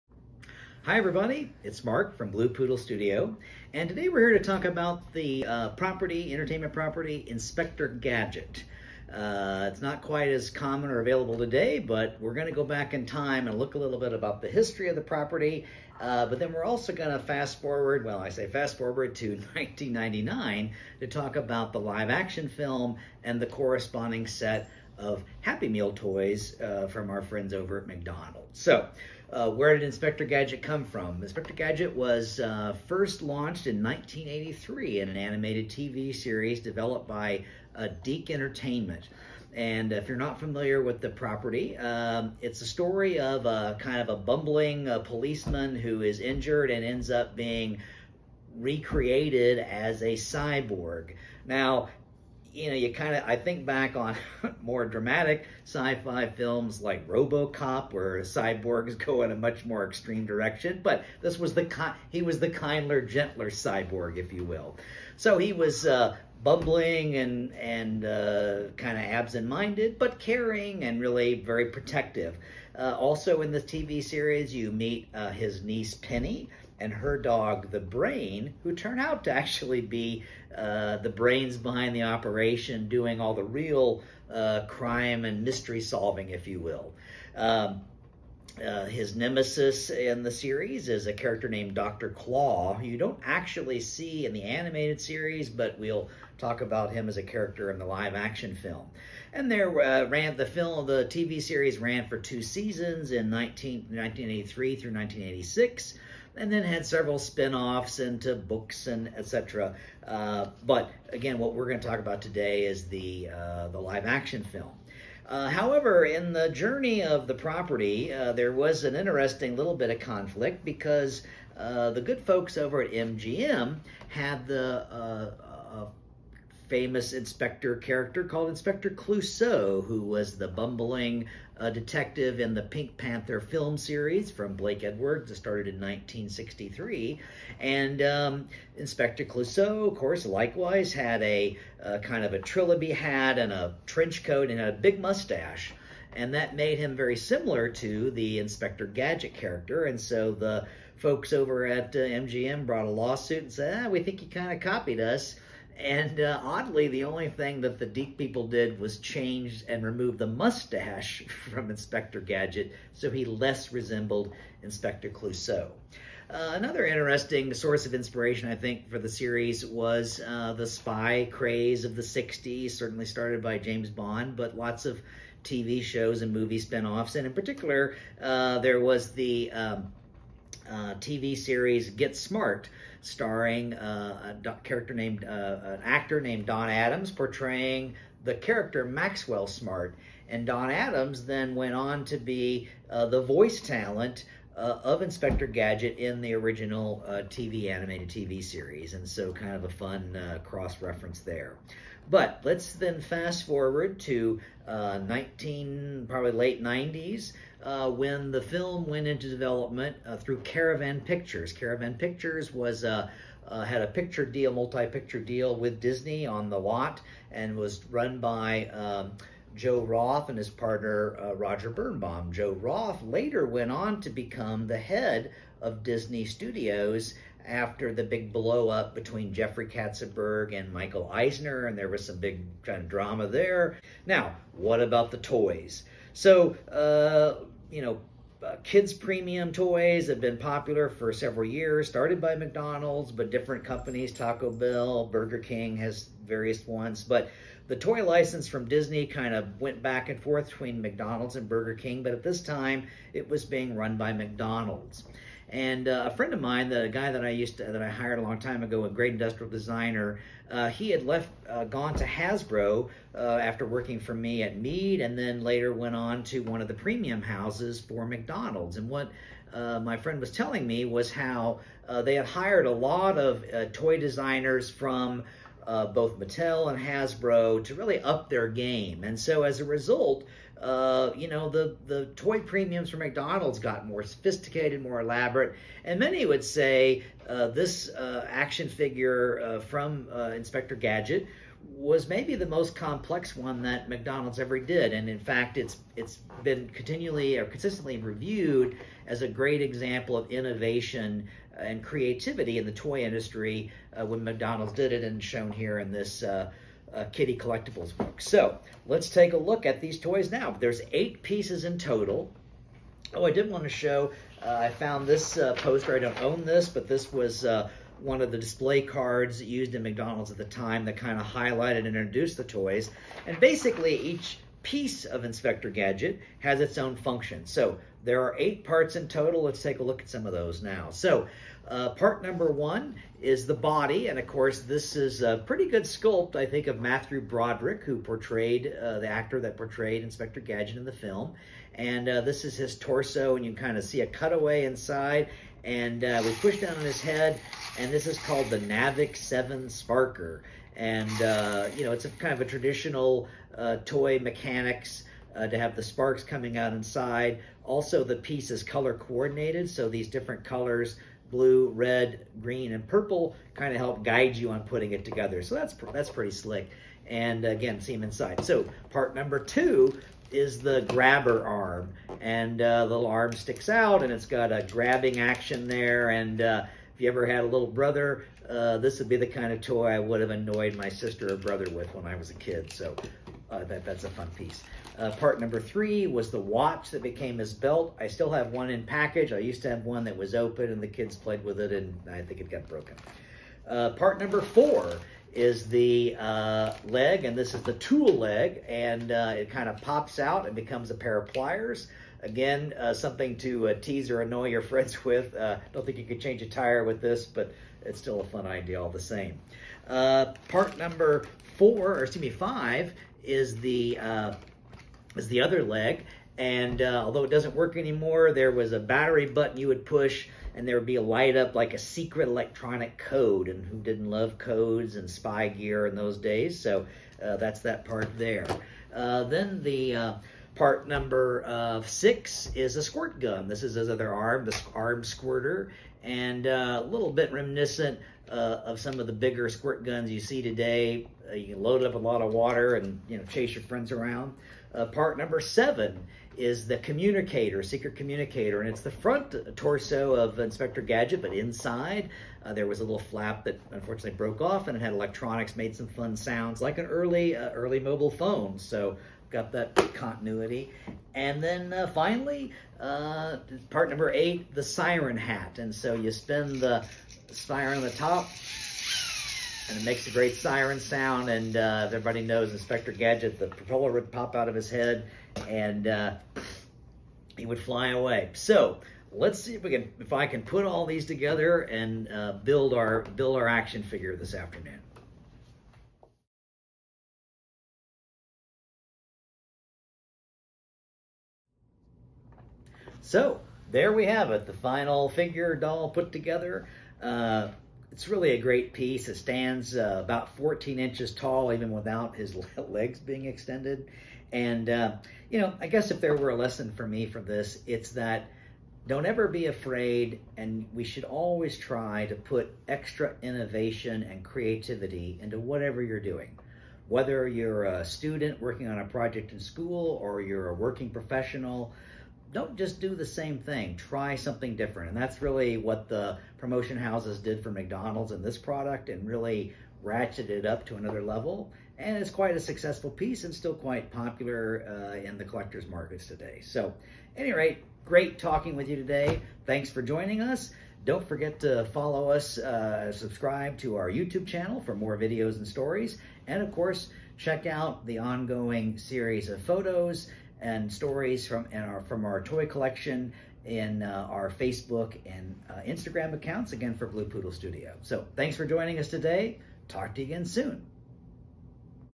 Toy Innovation from McDonald’s and Inspector Gadget
In this video Mark discusses the property Inspector Gadget and shows off the McDonald’s toys from 1999.